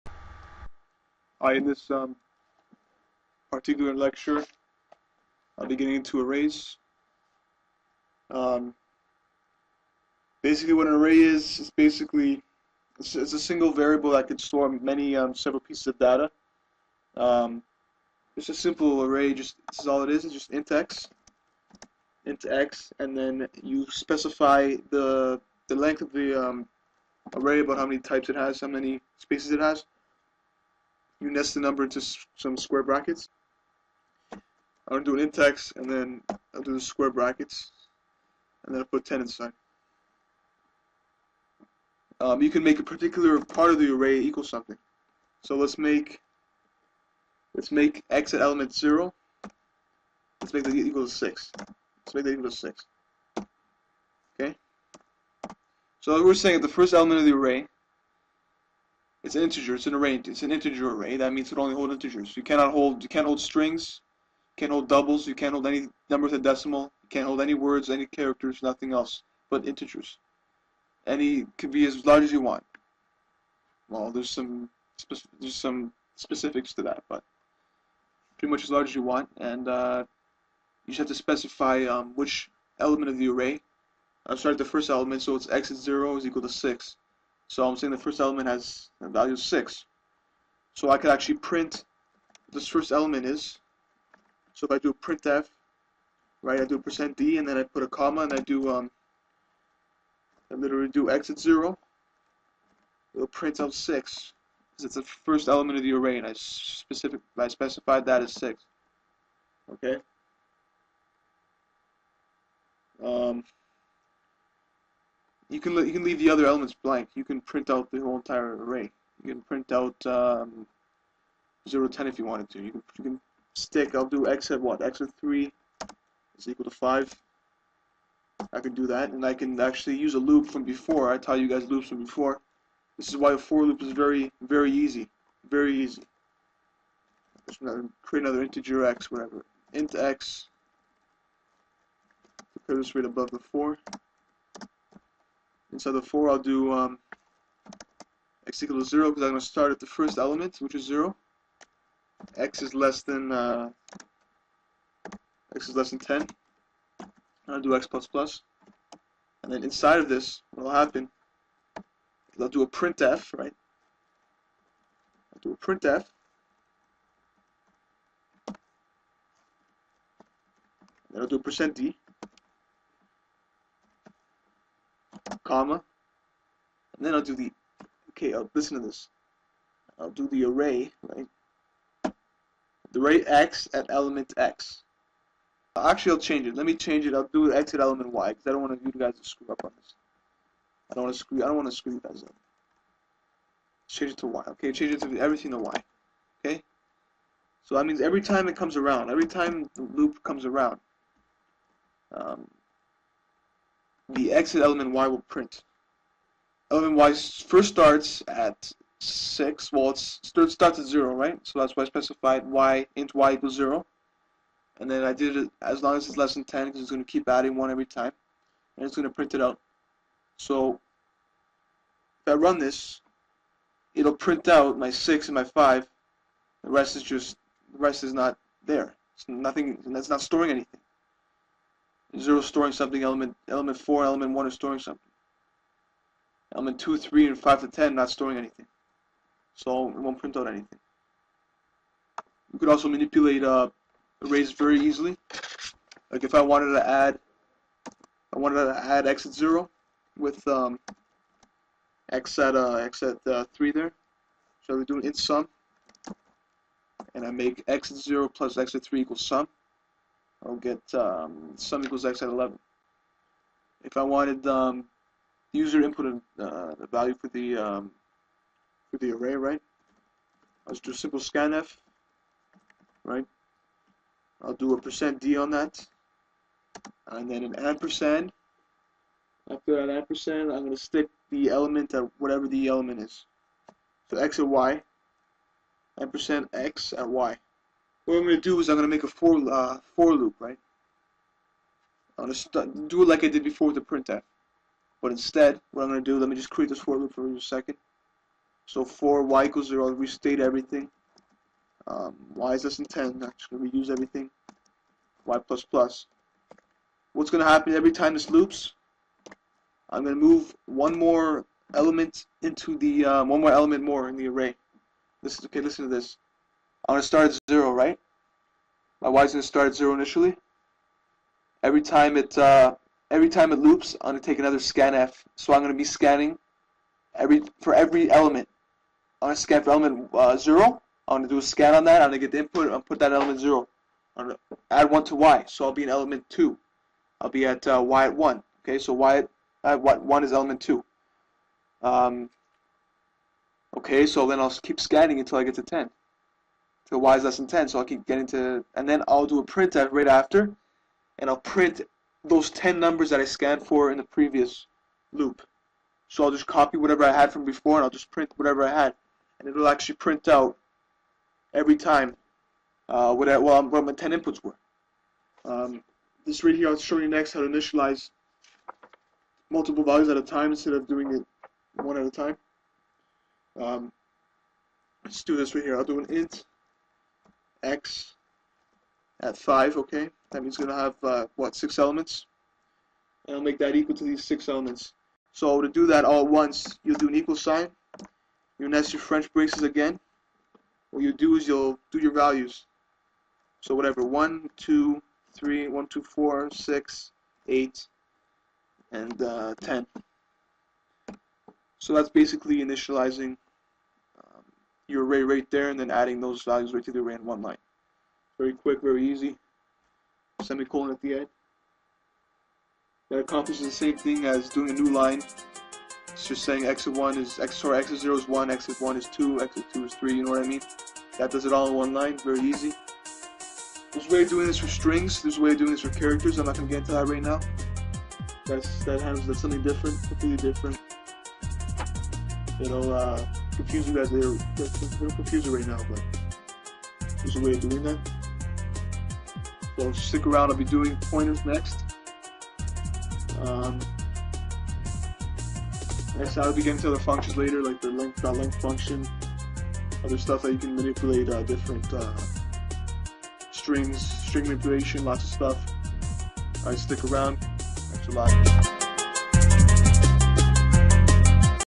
C Programming [4] - Arrays
In this tutorial I will show you the several different ways to create and set values to arrays as well as manipulating arrays through loops.